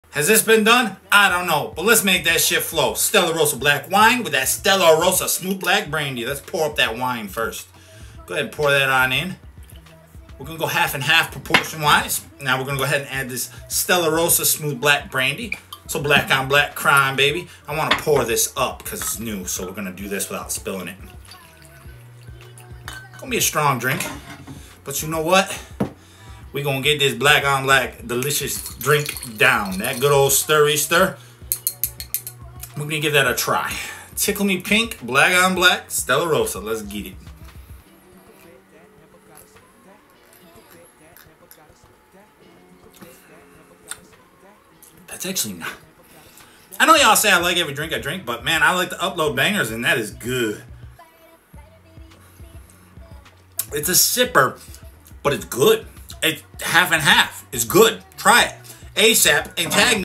Black On Black Crime 🤤🤤🤤
Black On Black 🤤🤤

This combo is for the ladies and fellas.  A straight banger!

Mixologist time!!!   Mixing up some

 Stella Rosa Black Wine
Stella Rosa Smooth Black Brandy